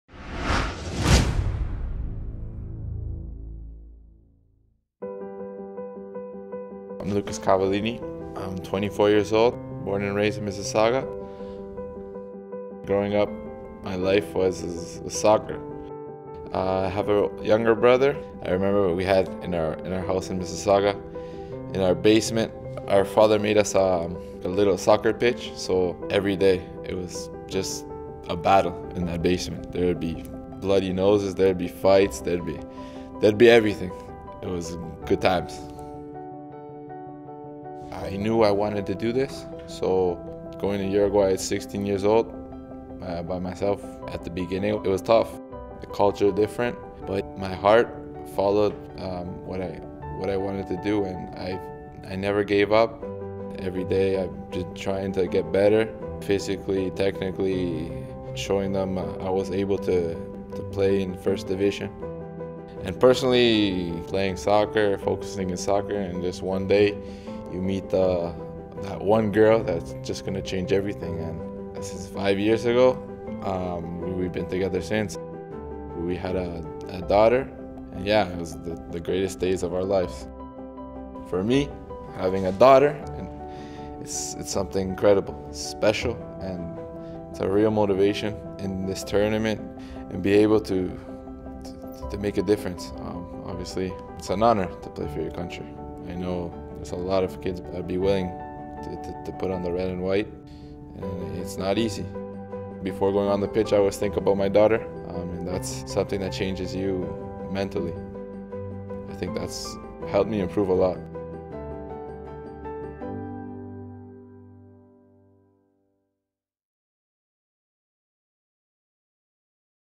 Canada Soccer Story: Lucas Cavallini
Meet 24-year-old CANMNT striker Lucas Cavallini who has been leading the line for Canada at the 2017 CONCACAF Gold Cup.